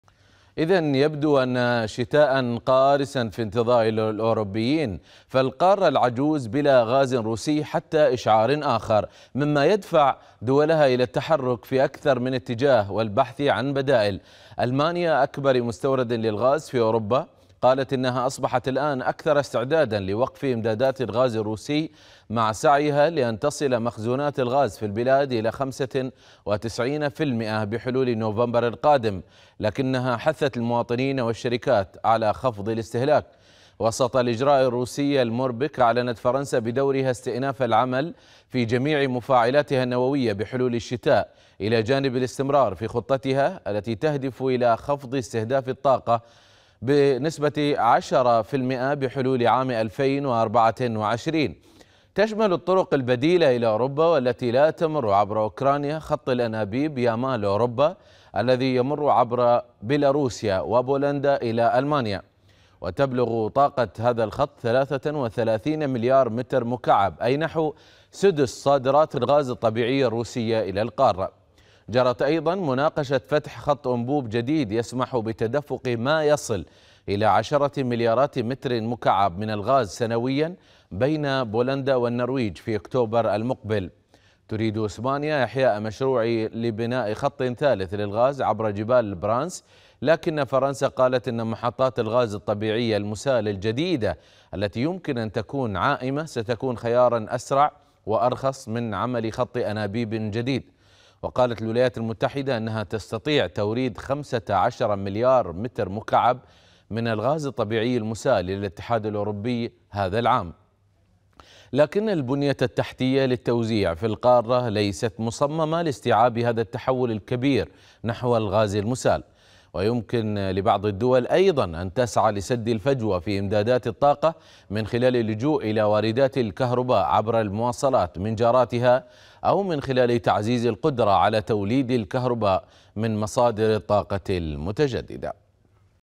أوروبا تغرق في المجهول بعد إغلاق خط الغاز الروسي نورد ستريم
إذا يبدو أن شتاء قارسا في انتظار الأوروبيين.. فالقارة العجوز بلا غاز روسي حتى إشعار آخر.. مما يدفع دولها إلى التحرك في أكثر من اتجاه، والبحث عن بدائل ..
ألمانيا أكبر مستورد للغاز في أوروبا، قالت إنها أصبحت الآن أكثر استعدادا لوقف إمدادات الغاز الروسي، مع سعيها لأن تصل مخزونات الغاز في البلاد إلى خمسة وتسعين في المئة بحلول نوفمبر القادم، لكنها حثت المواطنين والشركات على خفض الاستهلاك.